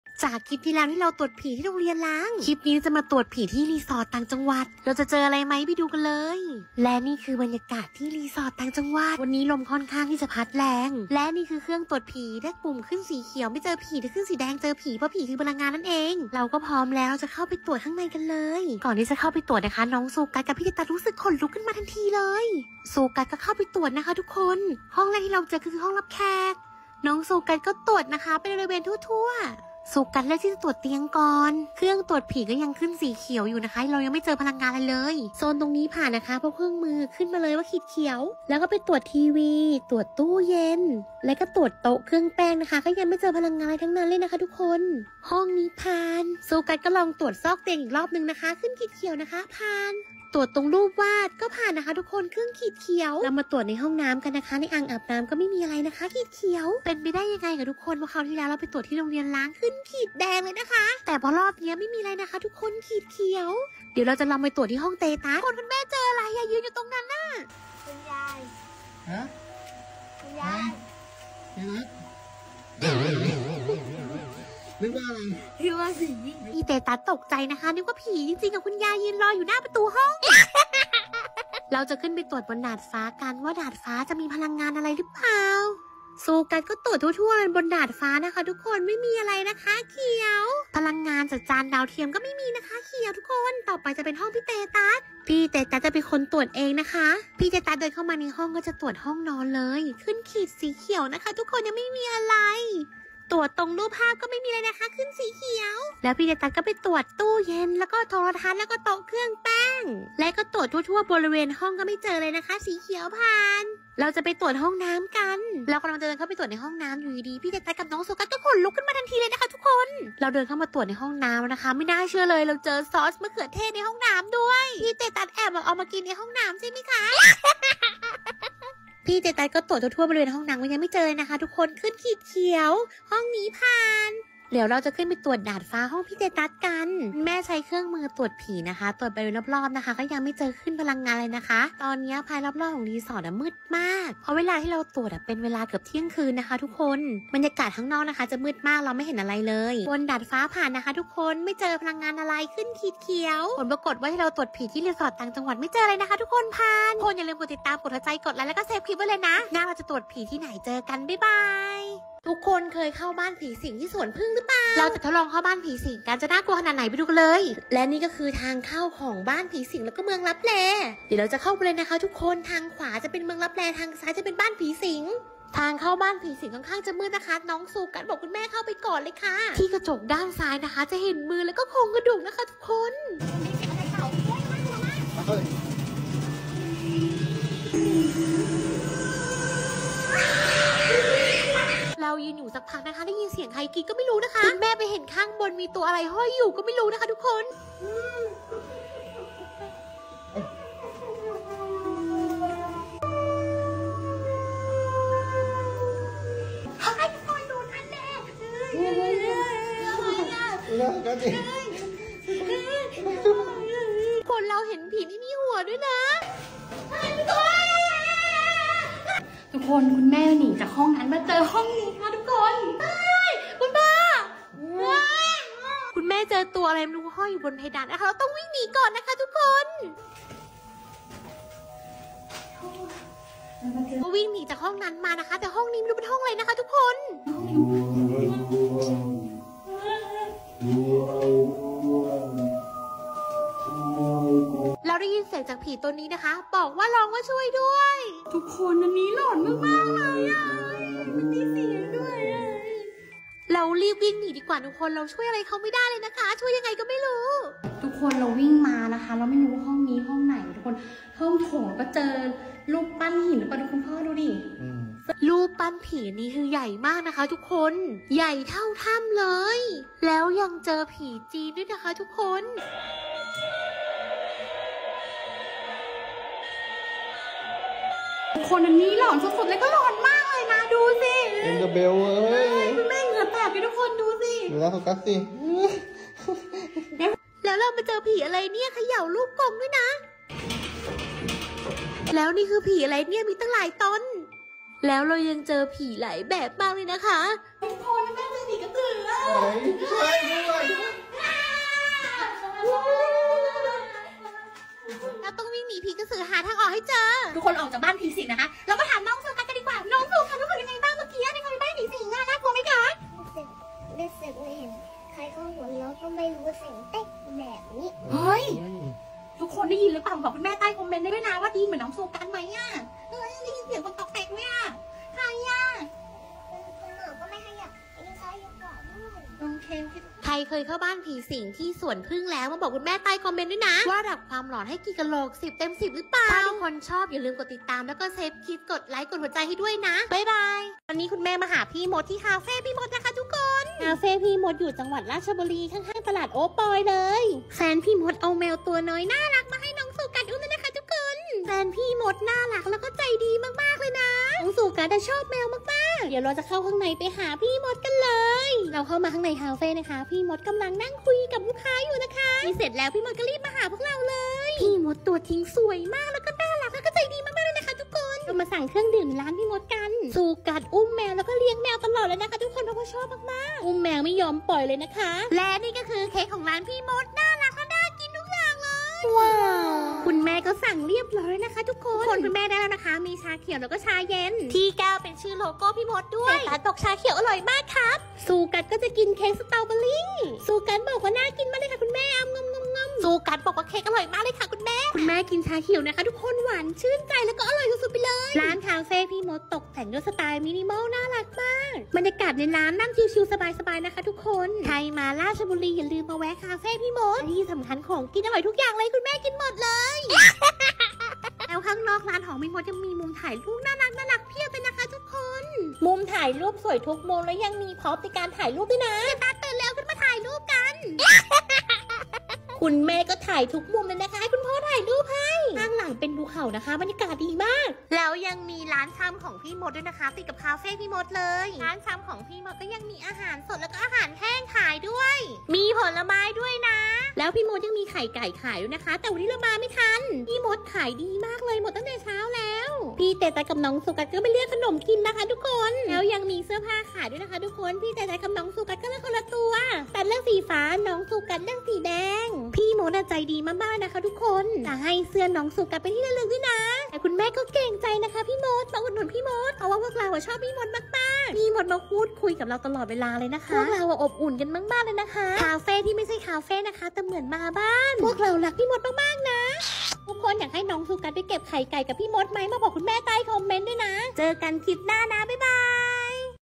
ทดลองตรวจผีที่รีสอร์ทต่างจังหวัด,ทดลองเข้าบ้านผีสิง,ซูกัสไปหาพี่มด
Fb        แม่สุน้องซูกัสMomsu sugus